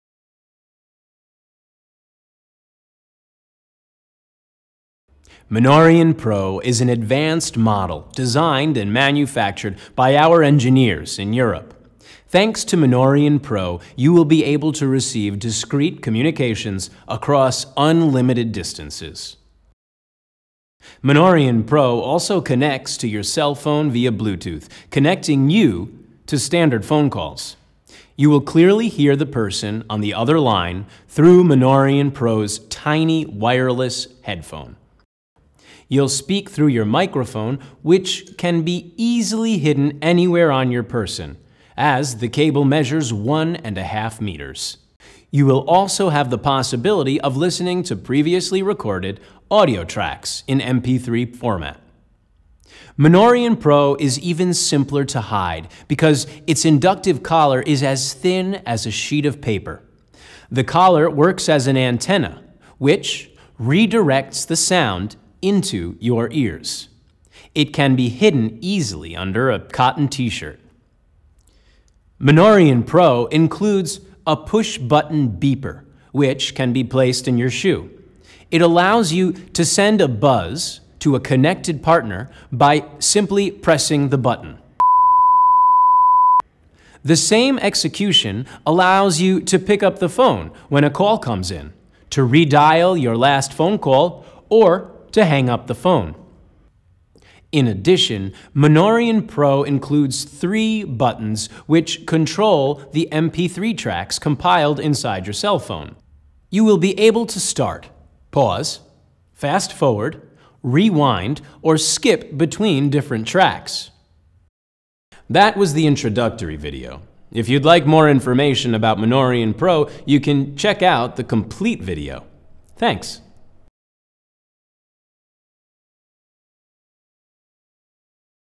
Monorean Pro — The Invisible Bluetooth Earpiece [SHORT VIDEO]
Buy the Monorean Pro wireless earpiece at our online store. The best wireless invisible earpiece.

Monorean Pro, the best Monorean!

Connect the invisible earpiece to your cell phone via Bluetooth and simply make a regular phone call.

Its inductive collar is as thin as a sheet of paper, and it won't even be noticeable under a thin cotton t-shirt.

Send buzzes to your partner and pick up the phone using the Beeper Pushbutton.

Control the MP3 files stored in your cell phone with the MP3 pushbuttons.

What's a Monorean?
The Monorean is a discrete communication device. The Monorean consists of an invisible earpiece the size of a rice grain that you put into your ear. Through the earpiece you’ll be able to listen to your partner crystal clear and in a discrete manner.

With the Monorean® Pro earpiece all set up, go to where you’ll use the earpiece..

• USE IN A PHONE CALL

Wait for an incoming call, and then pick it up by holding down the Pushbutton Beeper hidden in your shoe for five seconds. You can also press the Pushbutton Beeper to redial the last number called.

You’ll listen to the person on the other line through the invisible earpiece hidden in your ear

During a test, you can use the Pushbutton to communicate with your partner. For example: press once if you want to say 'yes', press twice for 'no', or press several times in a row to ask them to speak slower.

Hold the Pushbutton down for five seconds to hang up the phone.

• USE WITH MP3 PLAYER

Once you are ready to listen to the lessons stored in your cell phone, press the PLAY MP3 Pushbutton (you will have 3 Pushbuttons) to start listening to the audio tracks through the invisible earpiece hidden into your ear. Press the Pushbutton NEXT-TRACK to go to the next track. Press the Pushbutton PREVIOUS-TRACK to go to the previous track.

Hold down the Pushbutton NEXT-TRACK to fast forward and find a specific time on the track. You can also hold down the Pushbutton PREVIOUS-TRACK to rewind and find a specific point in your pre-recorded lesson.